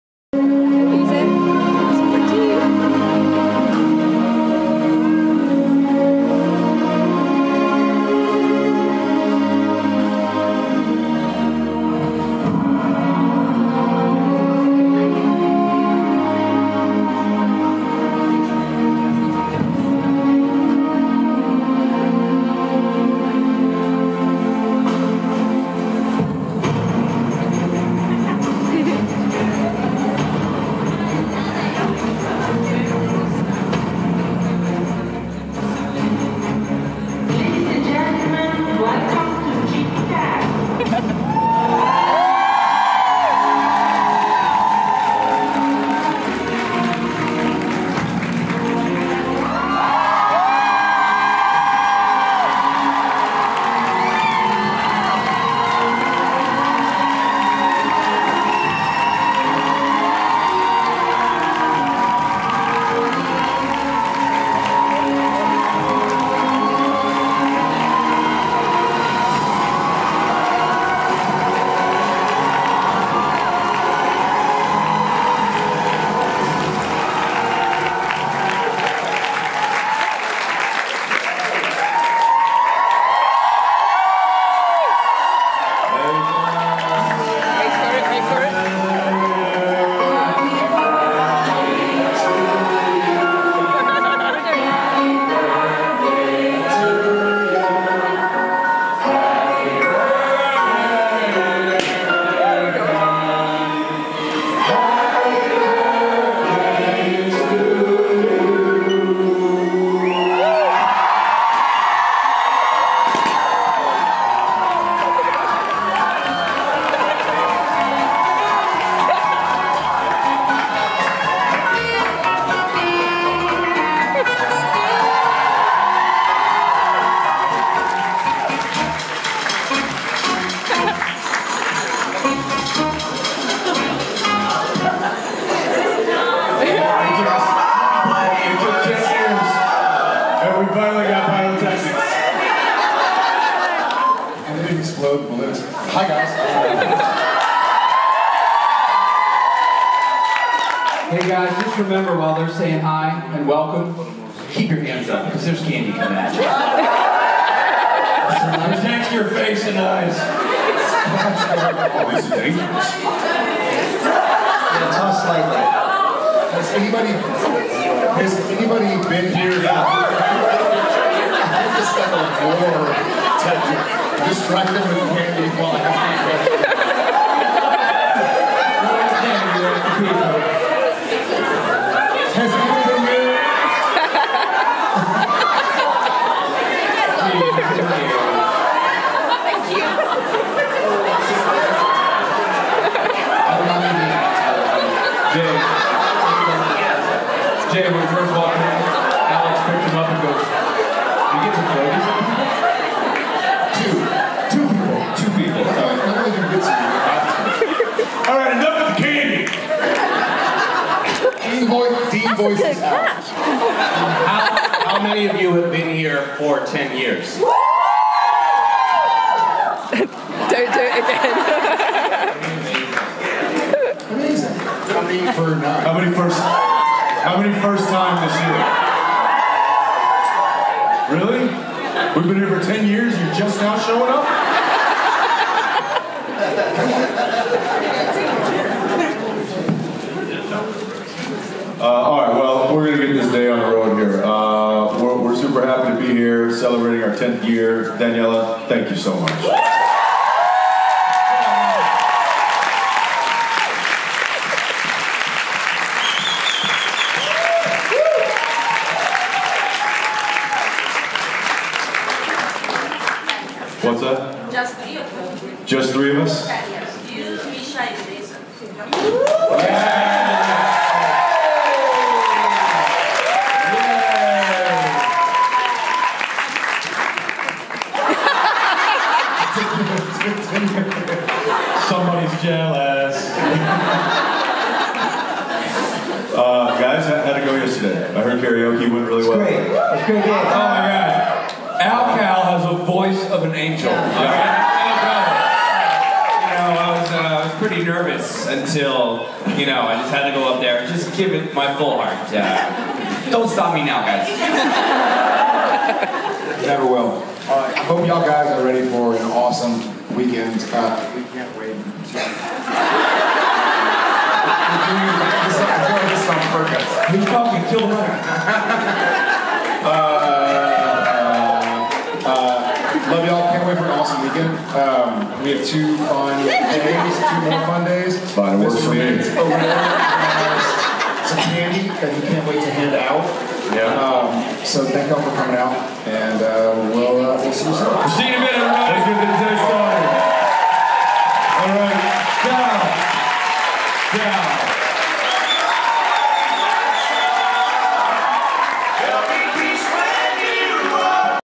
2019 JIB10 Opening ceremony
I admit, I'm proud of myself for managing to catch some of the candy while still filming - yeah, baby! Camera in one hand, snatching candy out of the air with the other. It must be my superpower.